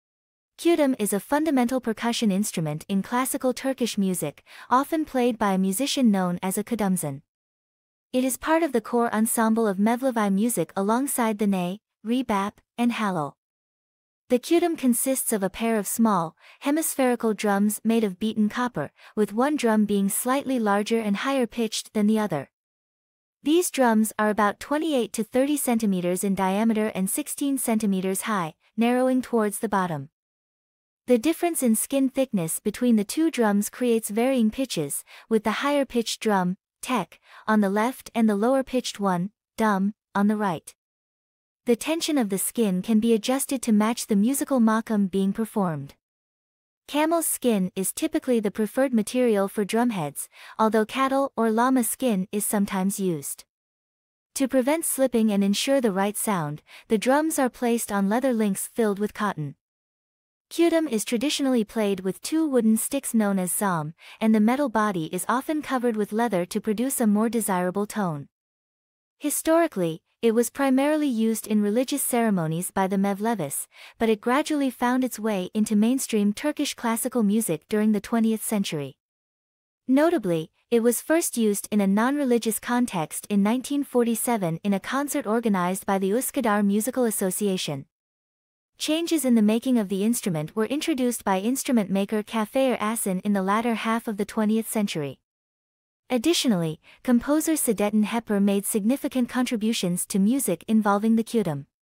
Kudüm - Turkish Classical Music Instruments
Kudüm is a fundamental percussion instrument in classical Turkish music, often played by a musician known as a kudümzen. It is part of the core ensemble of Mevlevi music alongside the ney, rebap, and halile. The kudüm consists of a pair of small, hemispherical drums made of beaten copper, with one drum being slightly larger and higher-pitched than the other. These drums are about 28-30 cm in diameter and 16 cm high, narrowing towards the bottom.

The difference in skin thickness between the two drums creates varying pitches, with the higher-pitched drum (tek) on the left and the lower-pitched one (düm) on the right. The tension of the skin can be adjusted to match the musical makam being performed. Camel's skin is typically the preferred material for drumheads, although cattle or llama skin is sometimes used. To prevent slipping and ensure the right sound, the drums are placed on leather links filled with cotton.

Kudüm is traditionally played with two wooden sticks known as zahme, and the metal body is often covered with leather to produce a more desirable tone. Historically, it was primarily used in religious ceremonies by the Mevlevis, but it gradually found its way into mainstream Turkish classical music during the 20th century. Notably, it was first used in a non-religious context in 1947 in a concert organized by the Üsküdar Musical Association. Changes in the making of the instrument were introduced by instrument maker Cafer Açın in the latter half of the 20th century. Additionally, composer Sadettin Heper made significant contributions to music involving the kudüm.